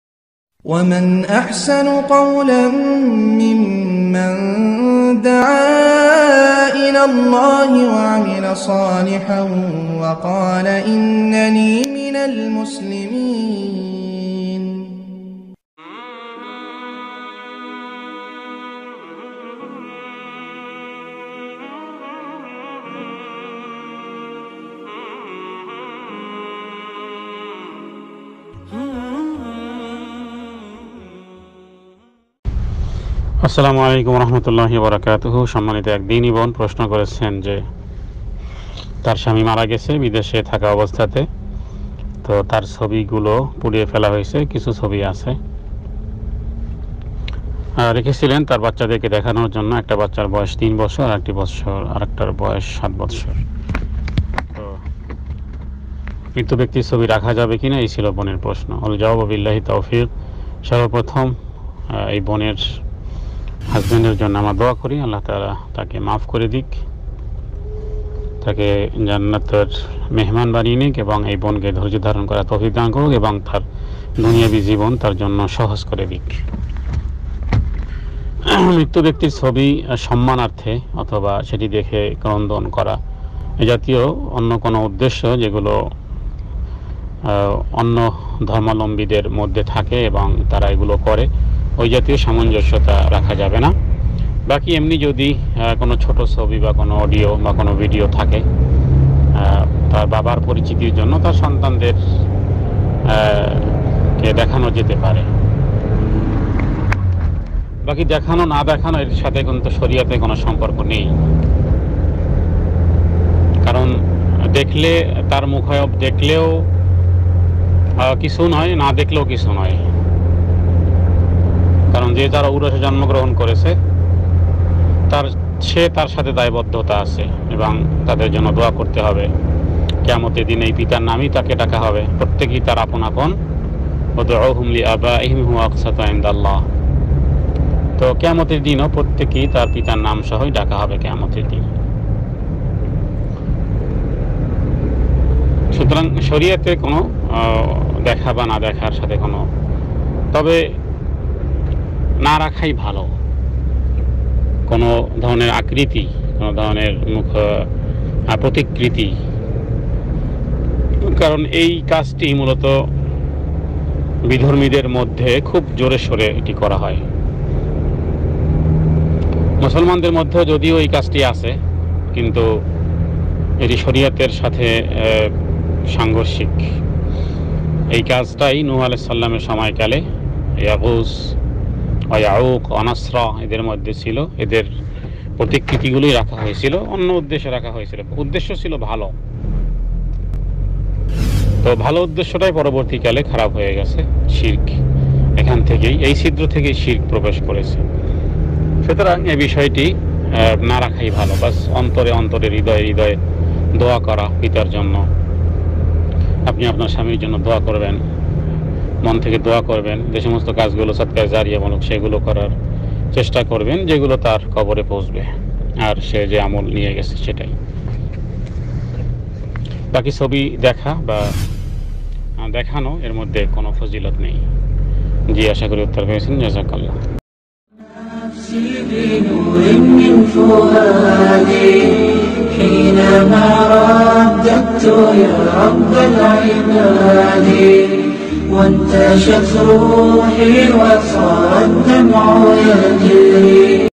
মৃত ব্যক্তির ছবি রাখা যাবে কিᴴᴰ┇by শাইখ মাসুম বিল্লাহ আল-আসাদ
মৃত ব্যক্তির ছবি রাখা যাবে কিᴴᴰ┇

What can be kept in the picture of the deceased person

🎙শাইখ মাসুম বিল্লাহ আল-আসাদ

🎙Sheikh Masum Billah Al-Assad.

👋 yeamul islam 👋